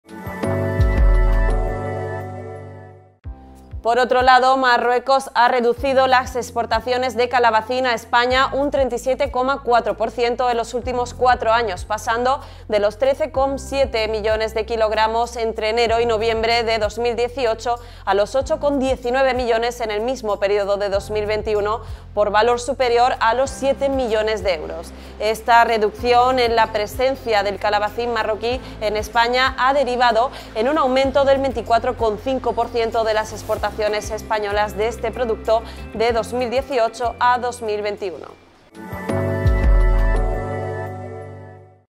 Marruecos reduce sus exportaciones de calabacines a España.
Marruecos ha reducido las exportaciones de calabacín a España un 37,4%, en los últimos cuatro años pasando de los 13,07 millones de kilogramos entre enero y noviembre de 2018, a los 8,19 millones en el mismo periodo de 2021 por un valor superior a los 7 millones de euros.
Esa reducción, en la presencia de calabacín marroquí en España, ha derivado en un aumento del 24,5% de las exportaciones españolas de este producto desde 2018 a 2021.